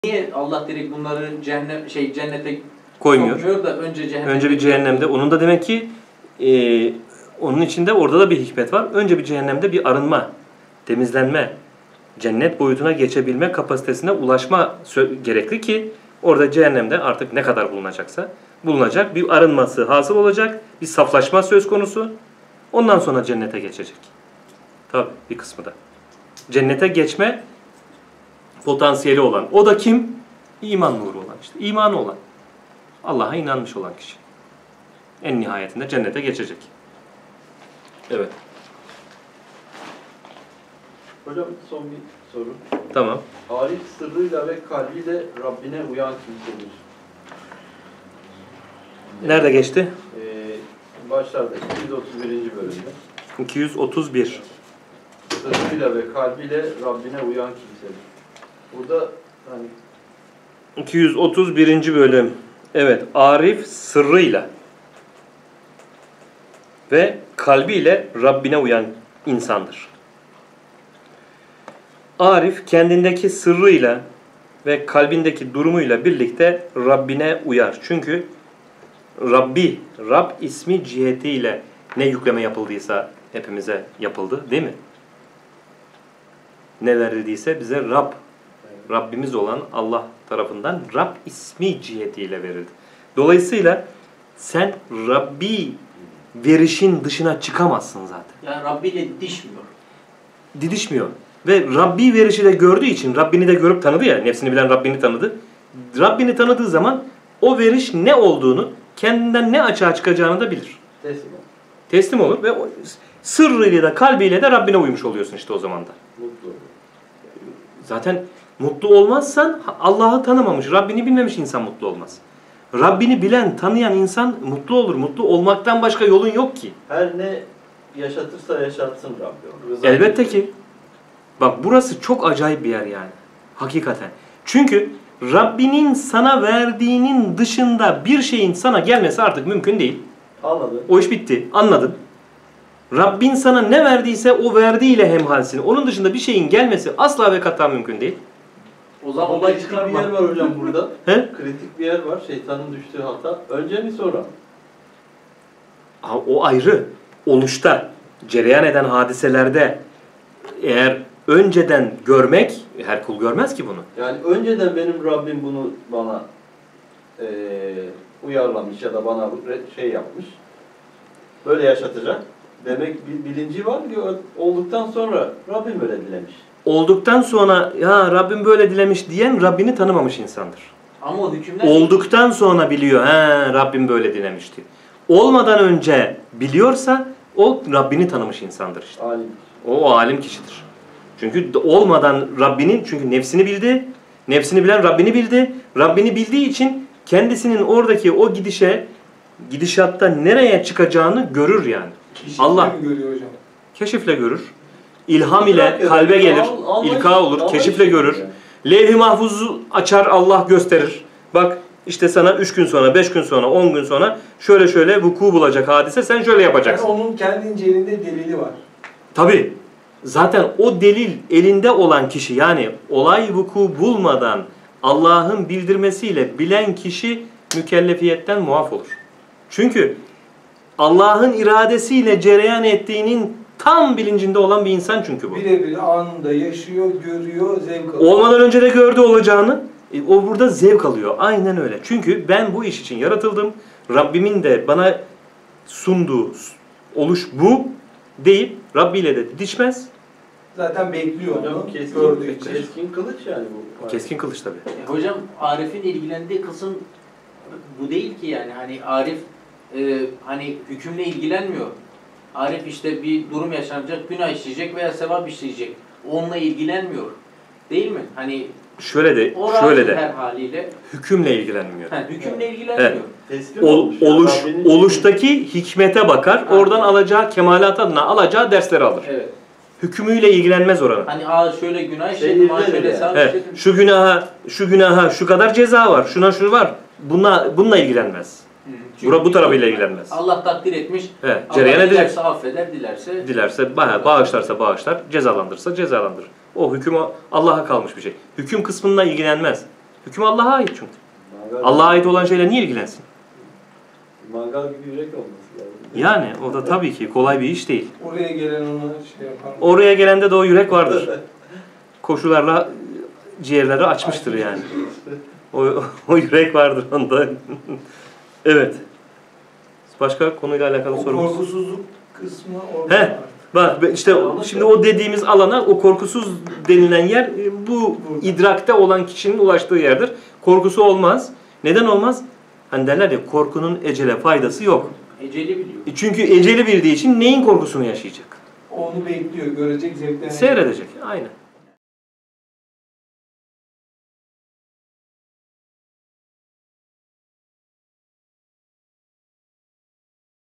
Rabbini tanıyan arifin durumu nasıl olur?
Not: Muhyiddin İbnü'l-Arabî Hazretleri (K.S)  Ekolü Üzere
Tasavvuf Sohbetidir.

Facebook: The School of Ibn Arabi

Arapça Yayın Yaptığımız Hesaplar